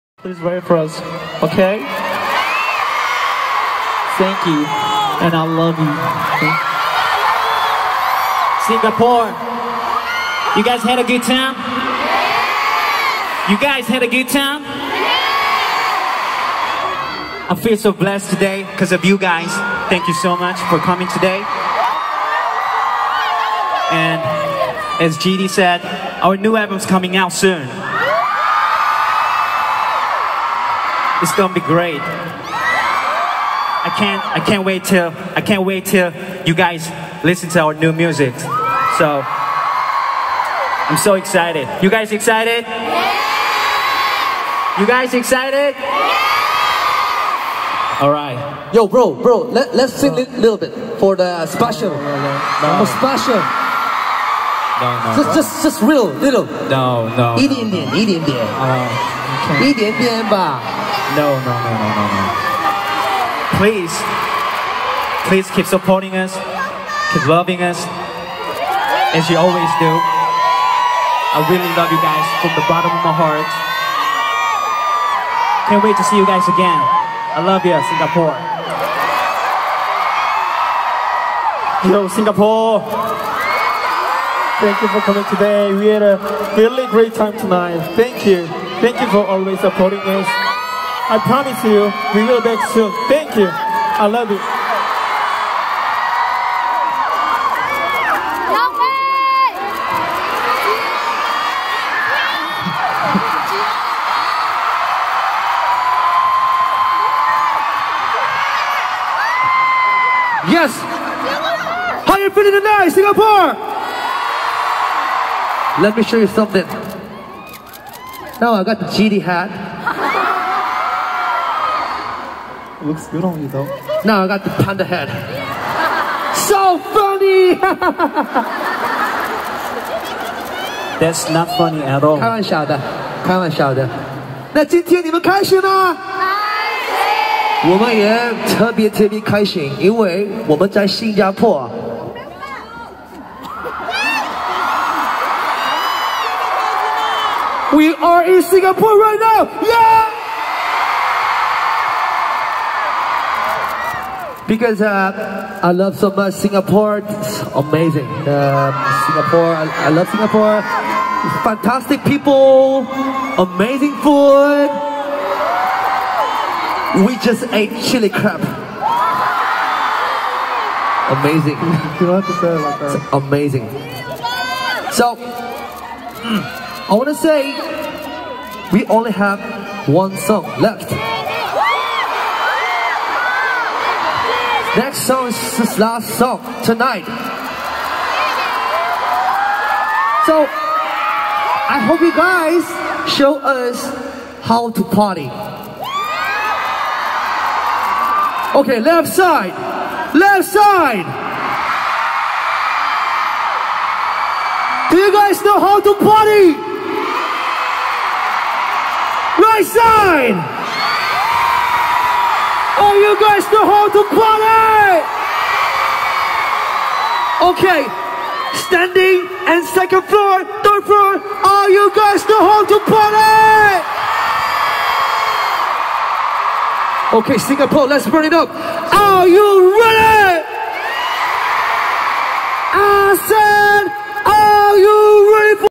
[HD] 161002 BIGBANG Talk cute G-Dragon focus - BIGBANG MADE VIP Tour in Singapore
GD wiping perspiration with a towel, slightly messy hair, cute and shy smile. GD focus fancam with a bonus Seungri in his Panda hat.